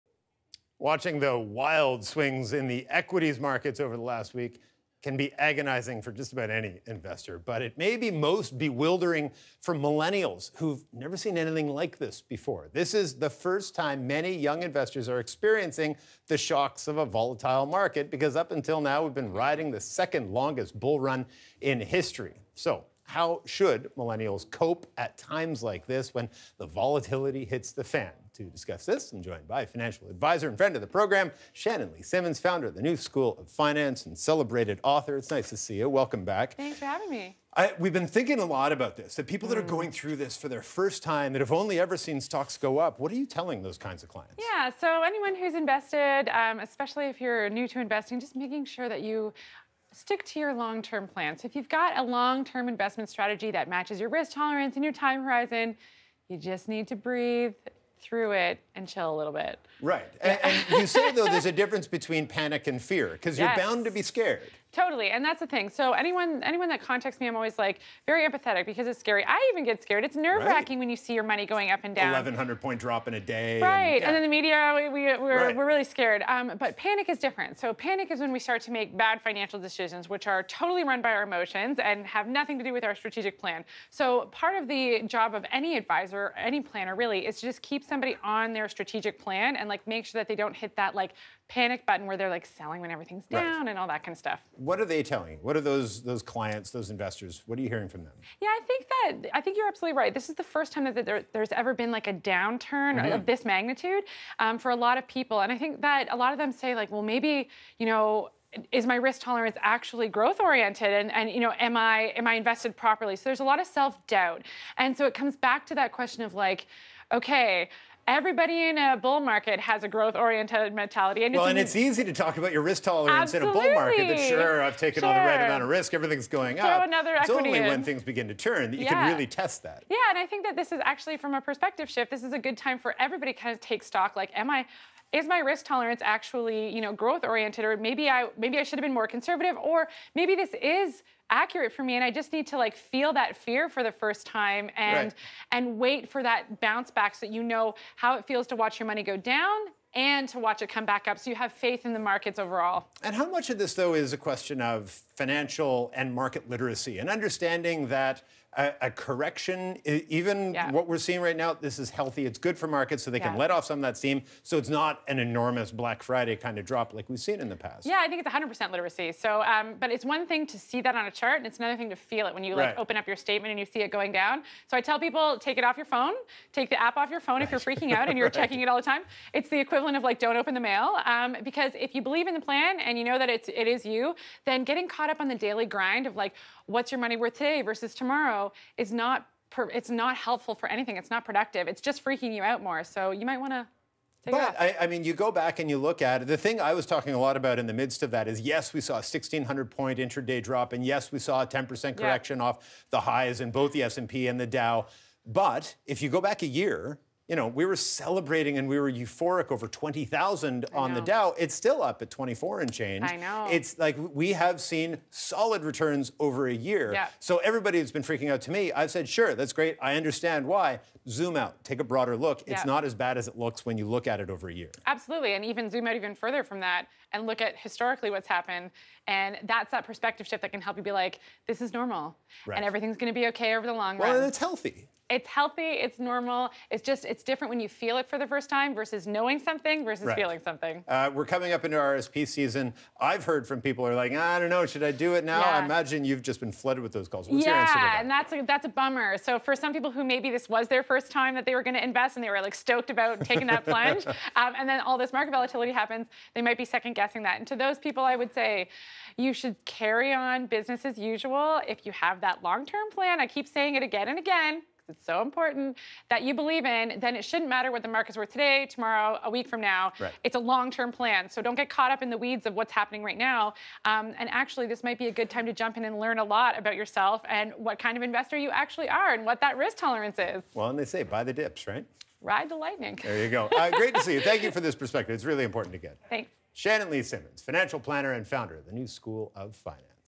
Investing for millennials: Don't panic
Millennial investors may be feeling the shocks of a volatile stock market for the first time, but experts say you should take a deep breath and stick to your strategy before deciding to sell.